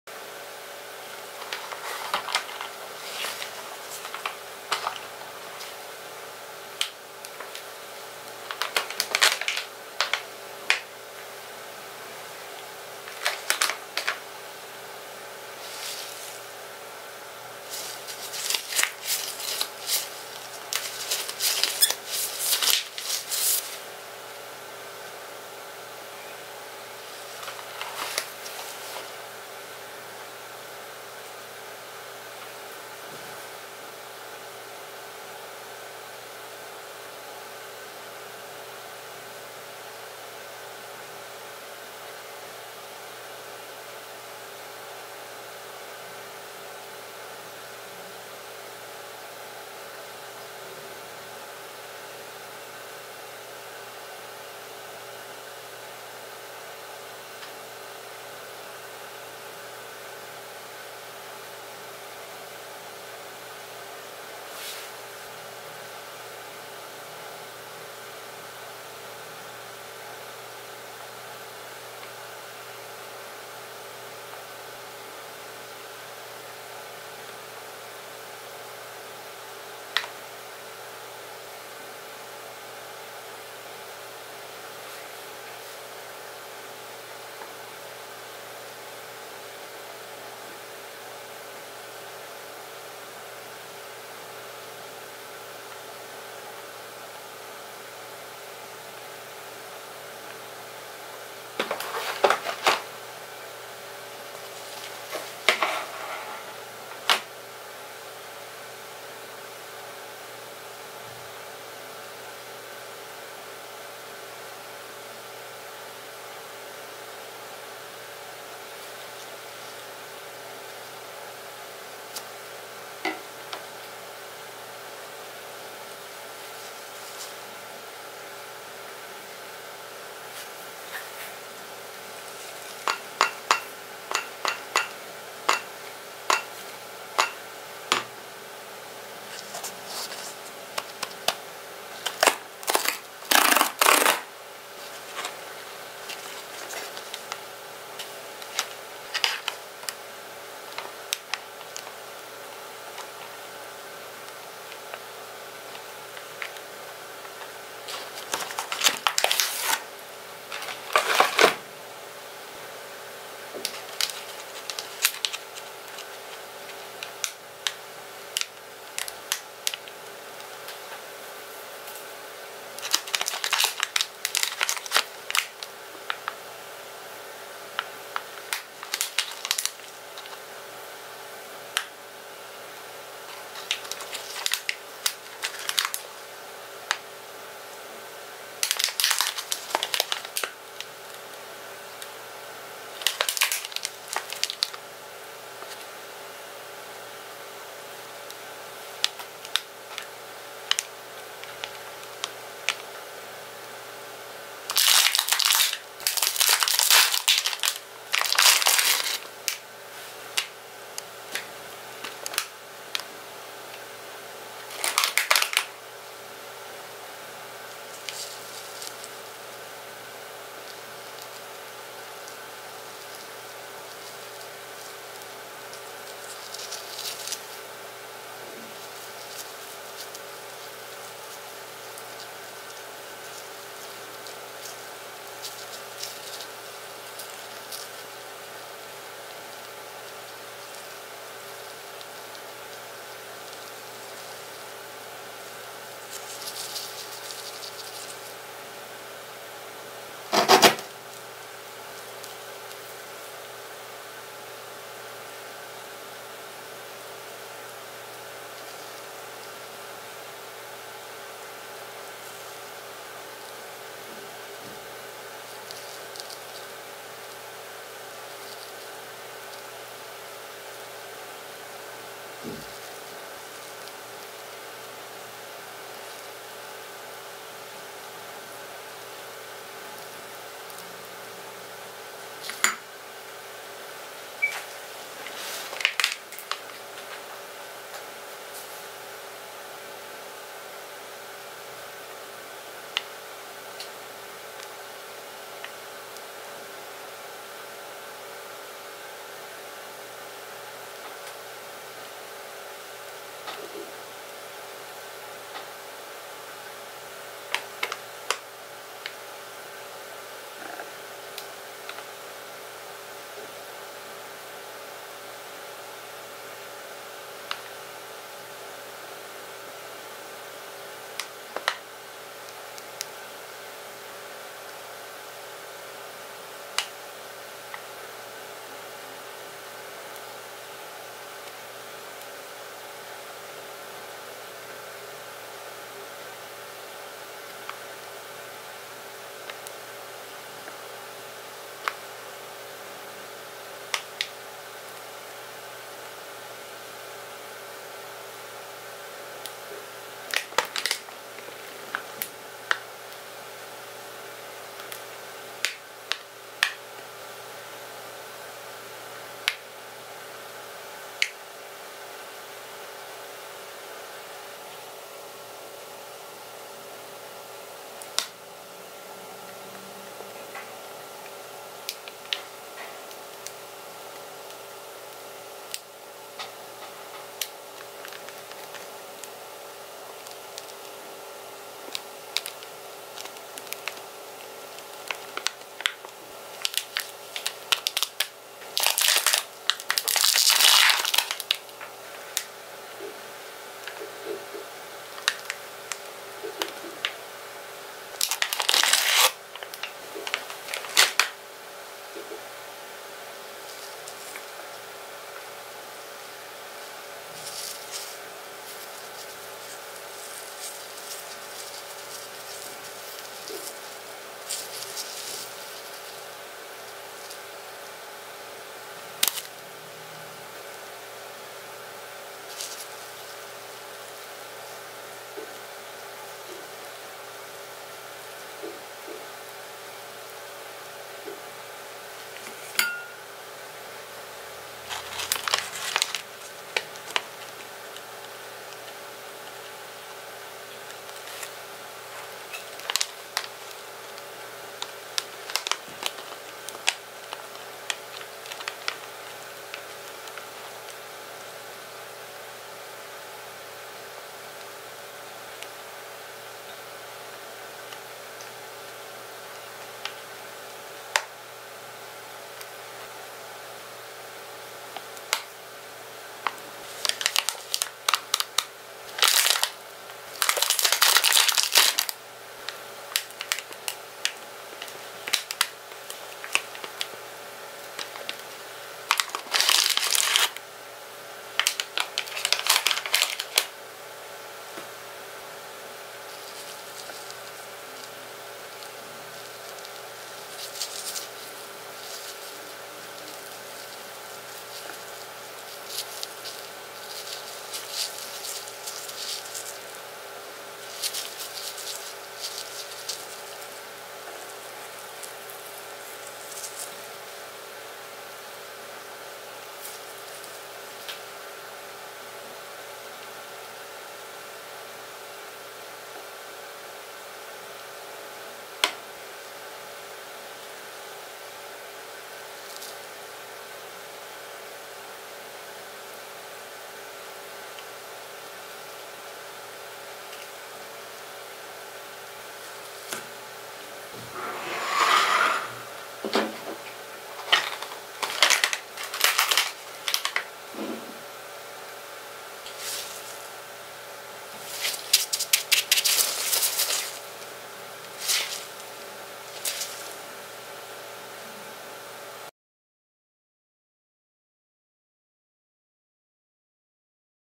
ASMR Unboxing Oreo Original The Batman
What do you think about Oreo Original The Batman?

Time-codes:
1. 0:00 - 0:25 - Intro
2. 0:26 - 4:43 - Big Oreo Original The Batman
3. 4:44 - 7:13 - Medium Oreo Original The Batman
4. 7:14 - 8:53 - Small Oreo Original The Batman
5. 8:54 - 9:17 - Completion